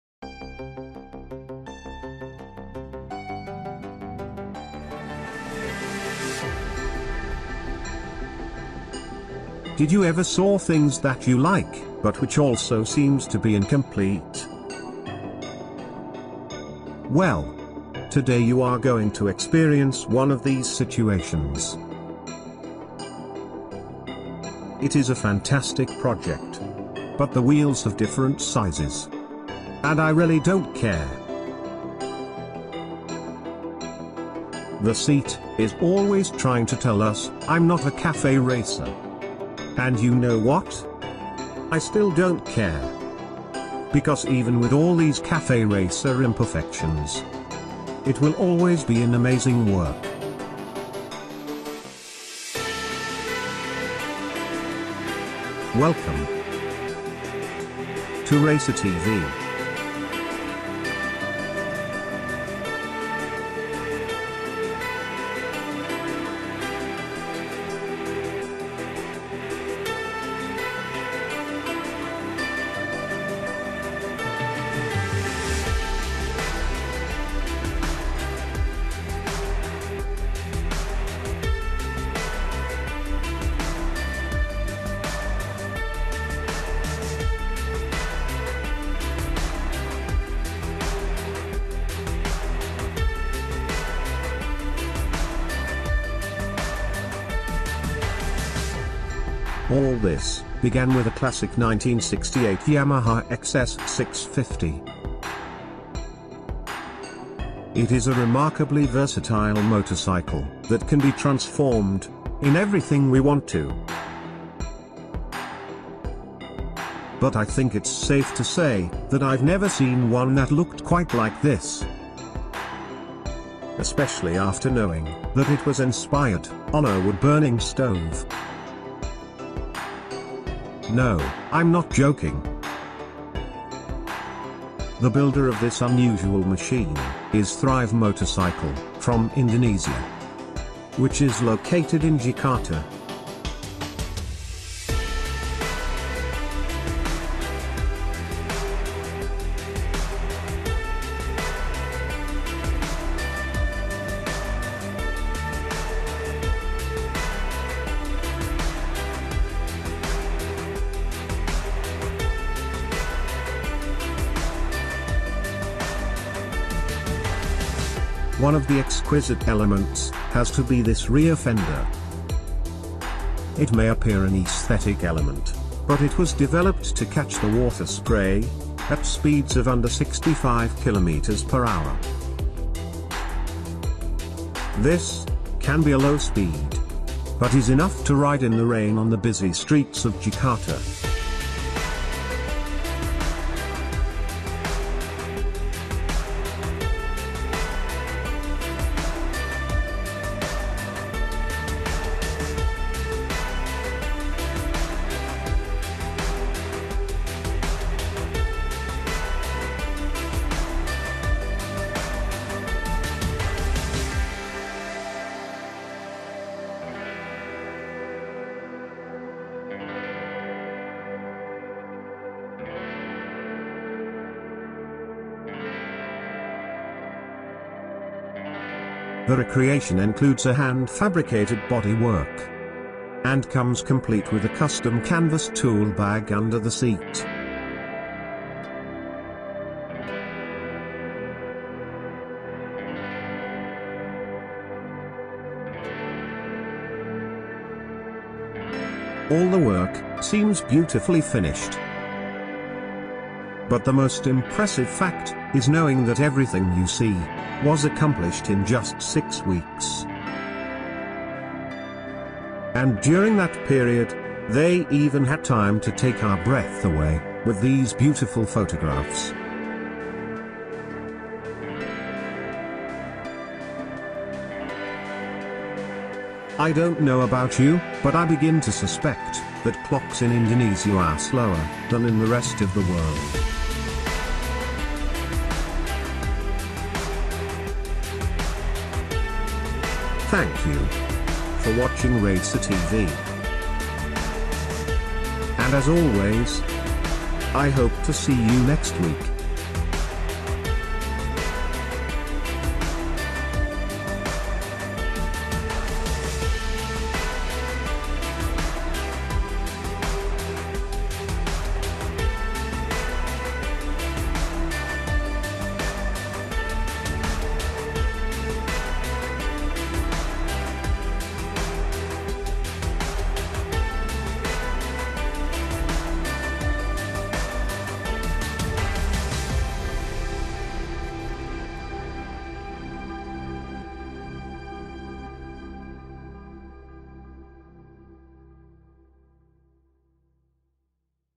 Cafe Racer (Yamaha XS 650 by Thrive Motorcycle)
If you ever saw things that you like, but which also seems to be incomplete, this bike can be one of these situations.
The Builder of this fantastic project, is Thrive Motorcycle located in Jakarta, from Indonesia.
It is based in a classic 1968 Yamaha XS 650.